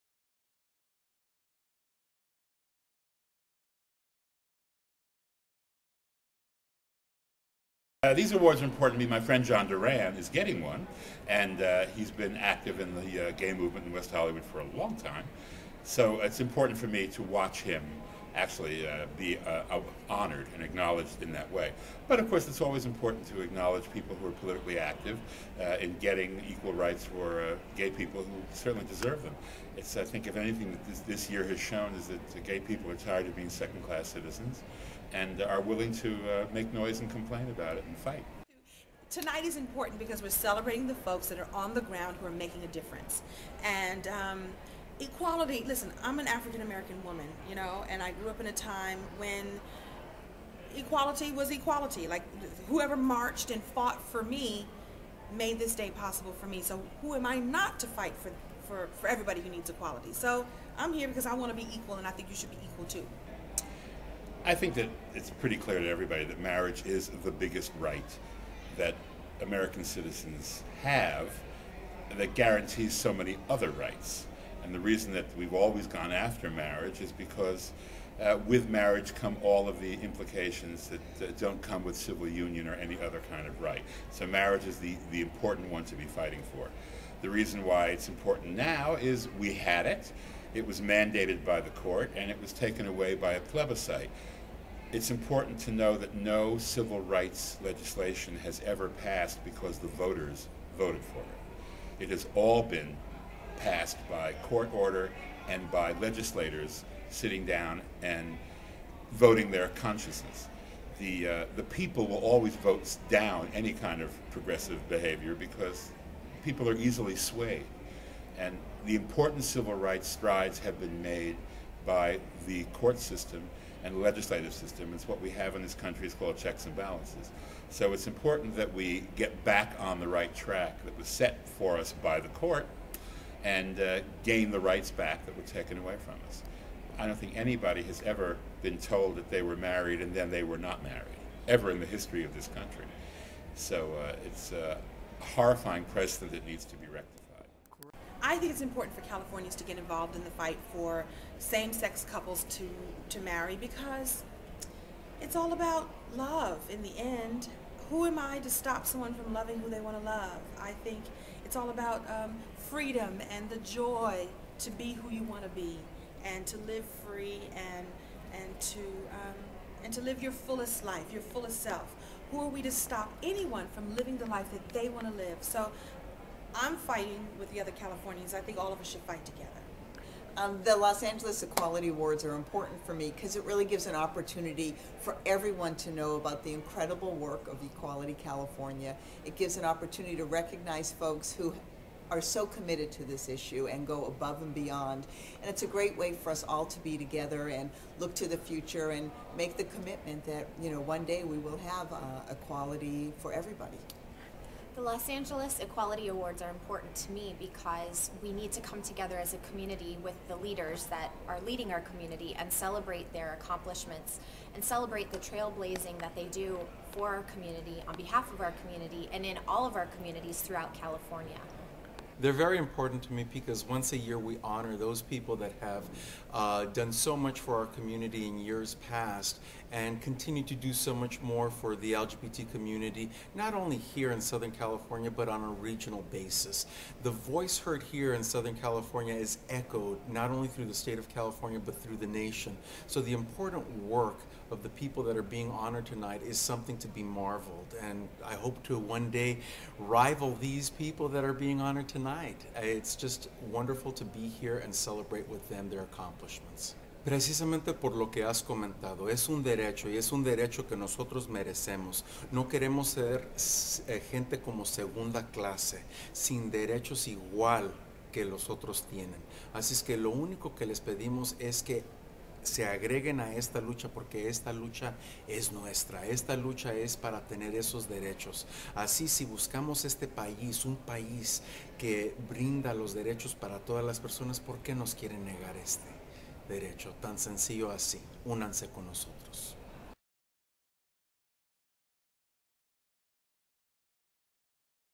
2009 Los Angeles Equality Awards
Presented by AT&T.
It's your night. Live it up.

We also put together this little video with remarks from some of the folks who attended, including comedians Bruce Villanch and Kim Coles, West Hollywood Mayor Abbe Land and City Councilmember Lindsay Horvath, and Juan R. Noguez, Vice Mayor of Huntington Park.